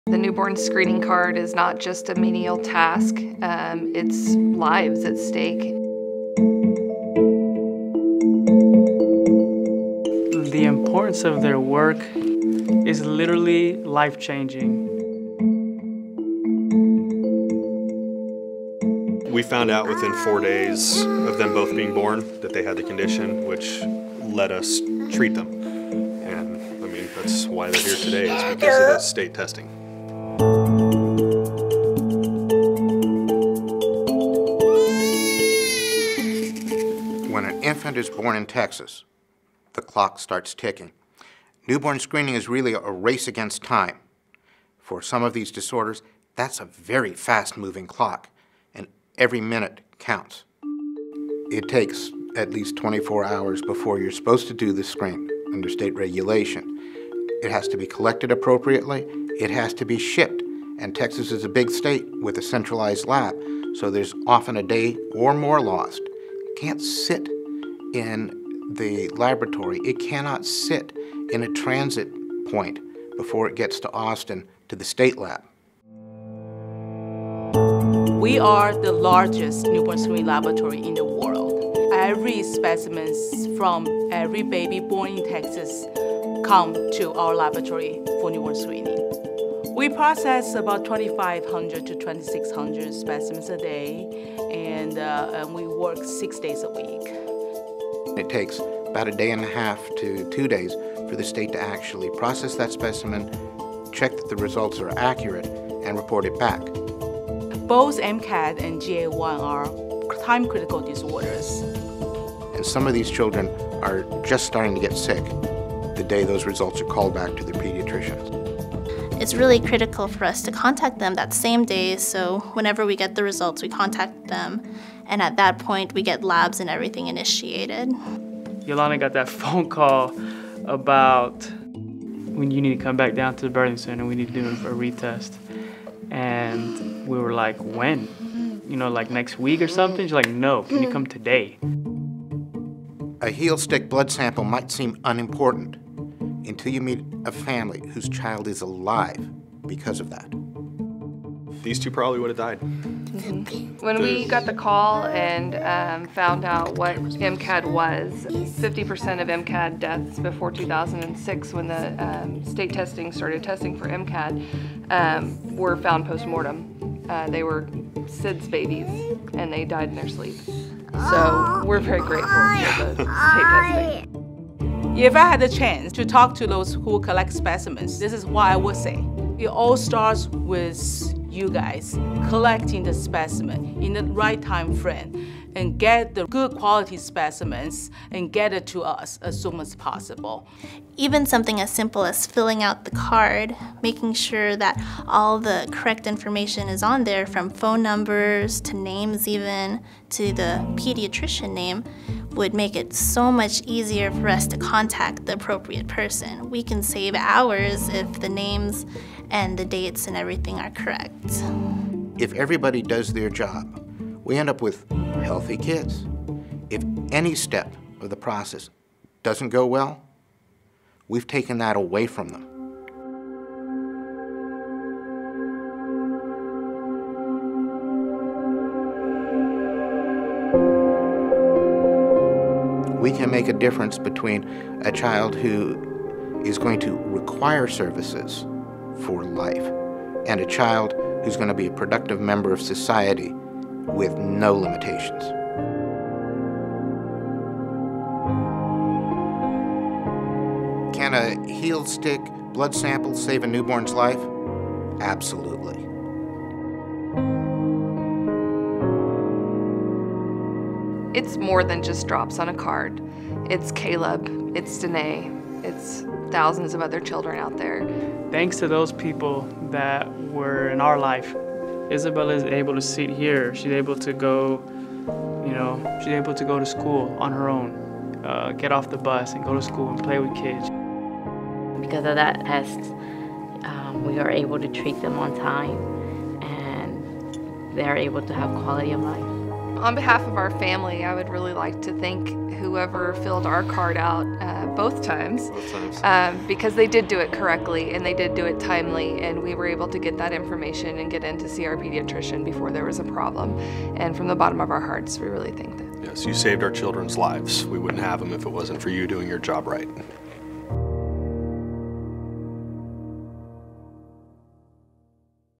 Newborn Screening More Than Drops On a Card
Texas newborns are screened for over 50 rare conditions through a blood test from a few drops on a card. For some of these conditions, every minute counts. It is critical that good quality specimens be collected in the right time frame and be shipped to the state lab as soon as possible.